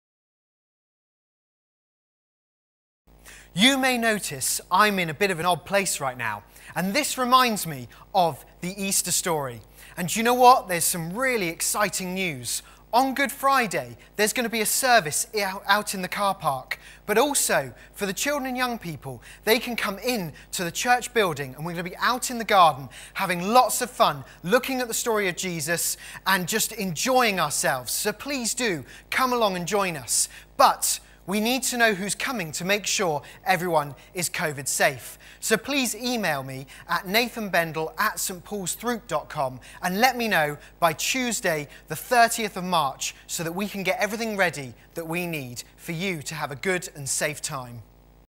Good Friday promo
We get to be together on Good Friday so please book on by emailing to say you and your children want to come by Tuesday 30th March, so that we can prepare resources in advance. Please be aware, parents must accompany their children throughout. Please also let us know of any food allergies.